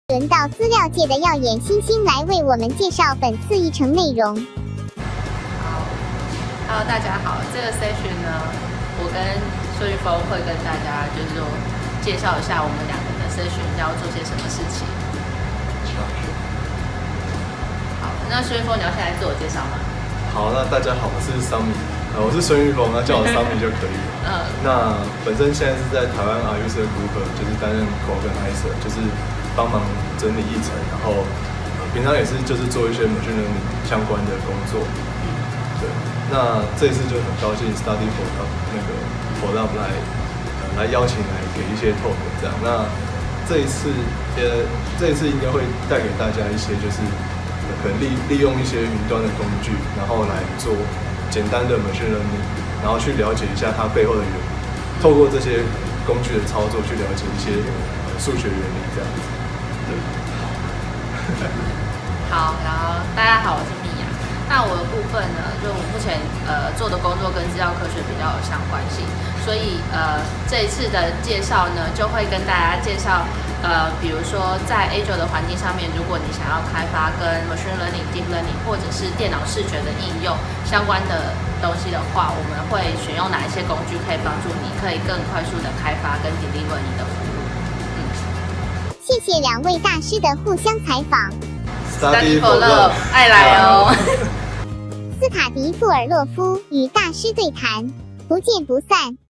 Study4Love 與大師對談 資料科學宣傳影片 - 張懷文 & 孫玉峰
『很多人說數據是資源，其實數據是土壤，下一代生意都在數據的基礎上長起來』 - 時間的朋友 2015
這波大數據浪潮襲來，你/妳準備好了嗎？
想讓自己快速成長，對數據有更深入的理解，可別錯過這場吸取大師濃縮精華的論壇啊～

Study4Love 與大師對談，即將在 2018/1/6 於台大舉行！

今天第一波講師對談，就由資料科學耀眼新星們 張懷文 & 孫玉峰 為我們開場！

首先，來點講師介紹：

🌟 張懷文 Mia

- Linker Networks 資料科學家
- Technofocus 和資策會的合作講師
- 微軟最有價值專家 MVP

"在 Azure 上開發電腦視覺相關應用 "
在 Azure 的環境上，如果想要開發 Machine Learning 或者電腦視覺應用相關，該選用哪些工具輔助你快速開發並且交付呢？

🌟 孫玉峰 Summit

- 雲深創新股份有限公司 研發總監
- Taiwan R user group 共同主持人

"大家都是十里坡劍神？談談資料科學的 - 從開始到放棄 - 如何從學習、試誤到從架構性思考應用與服務"
這次將會帶給大家，利用雲端工具來做簡單的 Machine Learning 機器學習，並且透過工具操作了解背後的數學原理！

讓我們看一下鏡頭裡講師們的羞澀介紹吧～
別忘了，如果妳/你期待這場活動，快分享給親朋好友們知道，一起揪團學習未來趨勢！🤗